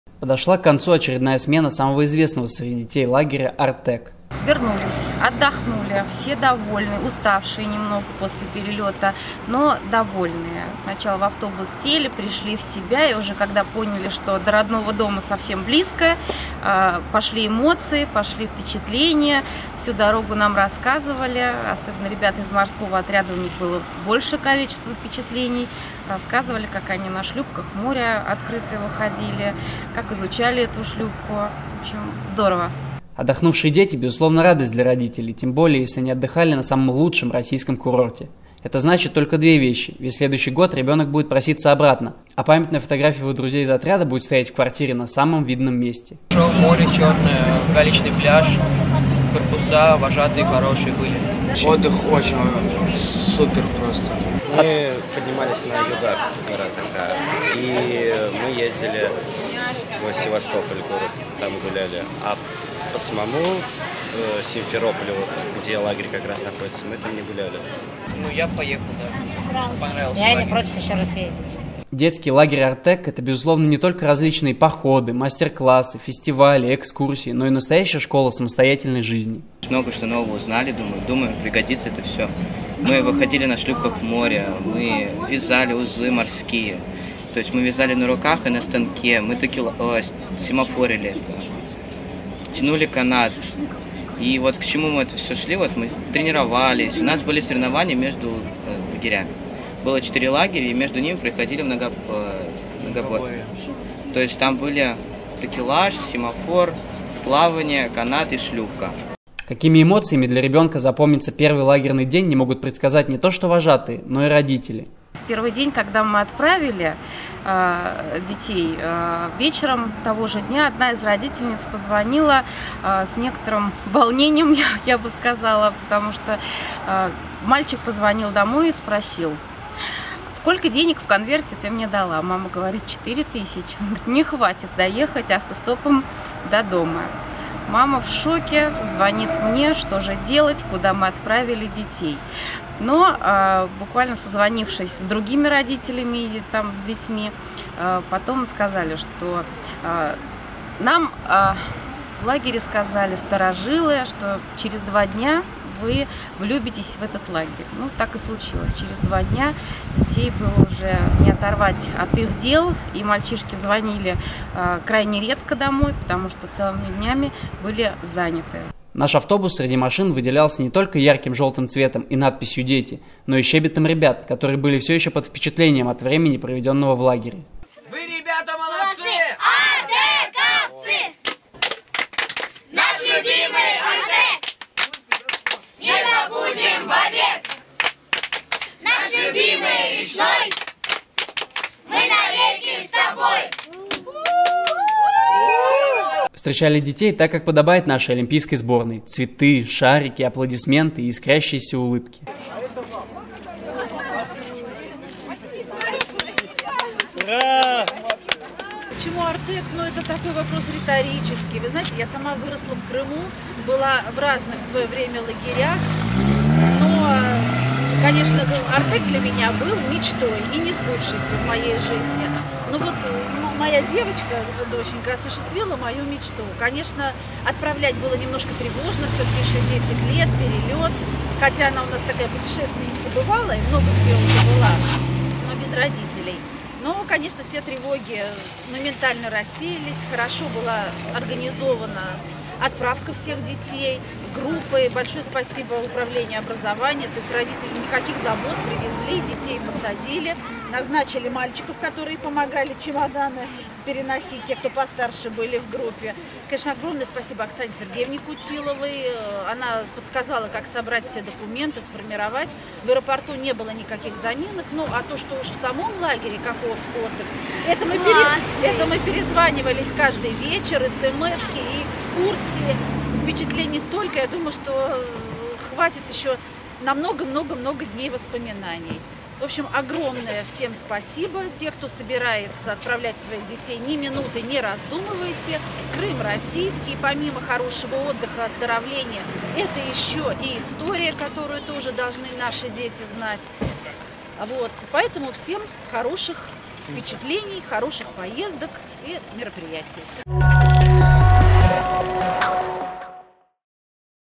Пионерский лагерь Артек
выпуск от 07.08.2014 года

Мы жители великой и многонациональной страны любим, когда к нам приходят в гости. А что уж говорить о том если в дом возвращается ребенок, которого не видел почти месяц. Конечно, радости нет предела. Ответственное и приятное поручение встретить детей с самолета и доставить их в руки скучавших родителей посчастливилось выполнить нашей съемочной группе.